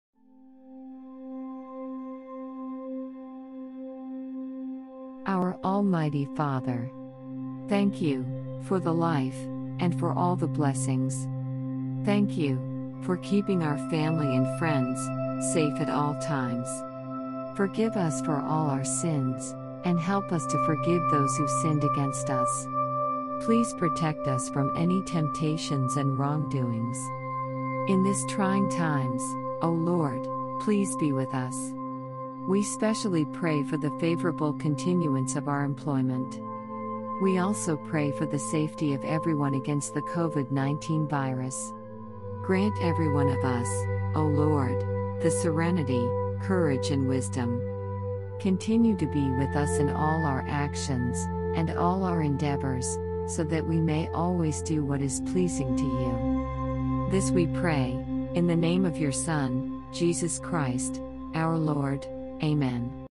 Prayer at the Start of Day (at work)
This is my current personal prayer.
This is a special prayer focused on the situation I am currently in.
I realized that I should have uploaded this before any other videos.

Note: If you want a personal prayer, you can comment your petitions and I will make a similar video so that you can play it anytime you want.

Edited with: KineMaster
Edited with: Narakeet
Production Assistant: JHL